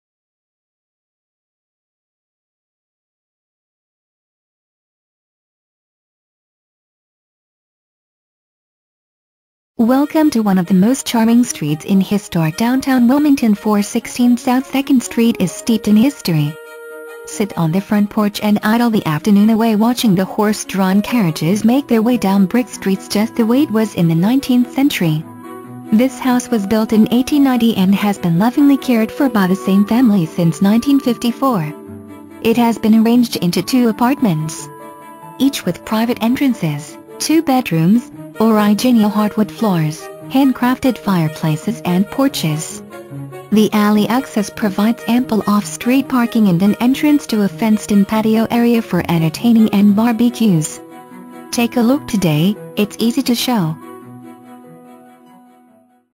416 S. 2nd Street
Welcome to one of the most charming streets in Historic Downtown Wilmington. 416 S. 2nd Street is steeped in history. Sit on the front porch and idle the afternoon away watching the horse drawn carriages make their way down brick streets just the way it was in the 19th century. This house was built in 1890 and has been lovingly cared for by the same family since 1954. It has been arranged into two apartments; each with private entrances, 2 bedrooms, originial hardwood floors, handcrafted fireplaces and porches. The alley access provides ample off street parking and an entrance to a fenced in patio area for entertaining and bar-b-ques. Take a look today, it's easy to show.

Presented By:
Mary M. Bridges, Prudential Laney Real Estate